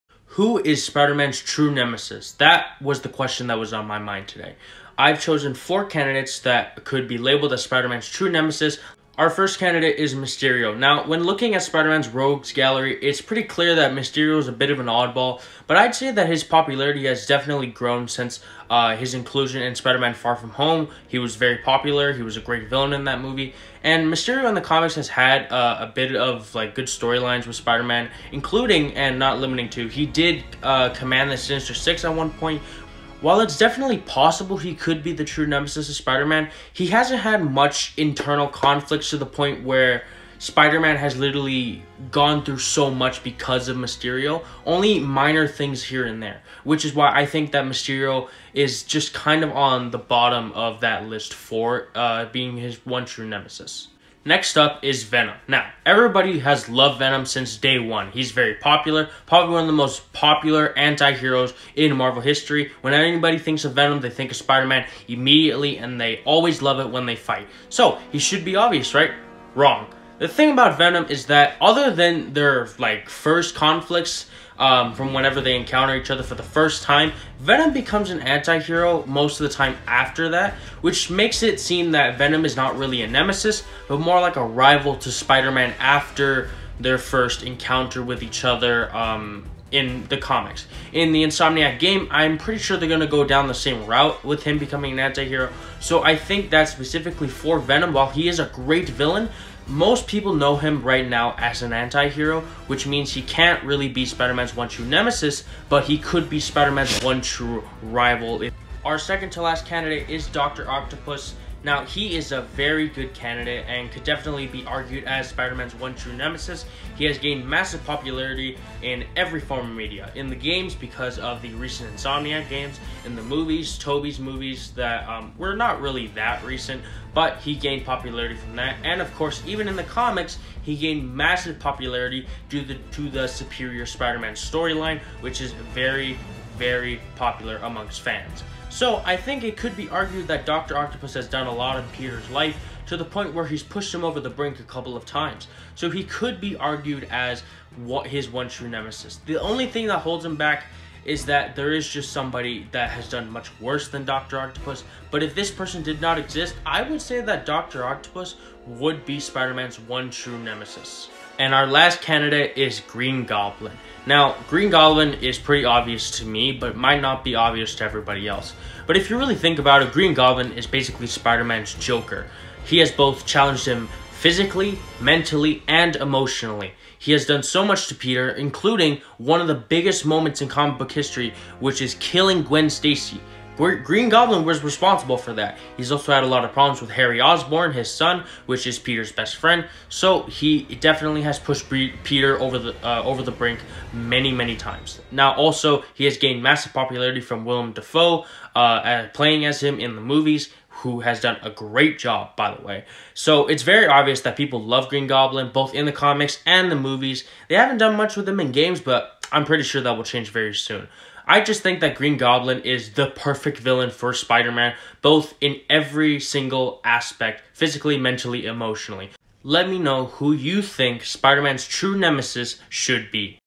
Who Is Spider-Man’s One True Nemesis?
Today we dive into an age old question, who is Spider-Man’s one true nemesis?